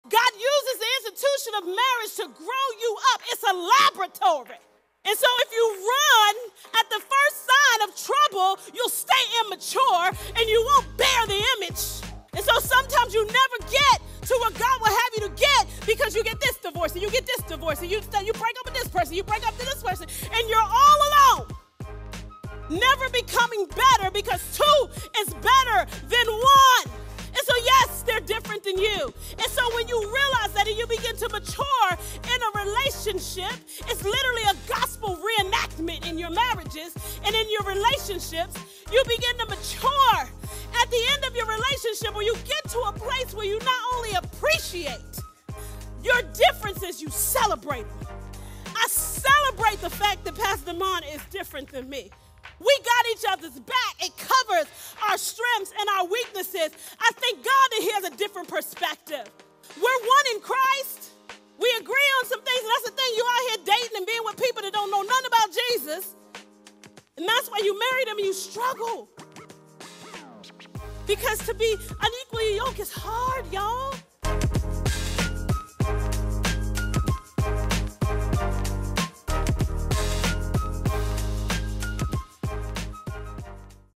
Your relationships should be maturing you! 🌱
Pastor Portia dropped so many gems and nuggets to help us with our relationships! Go to our YouTube Channel and check out the message Purpose Partners | February 11, 2024  to gain some wisdom for your relationship.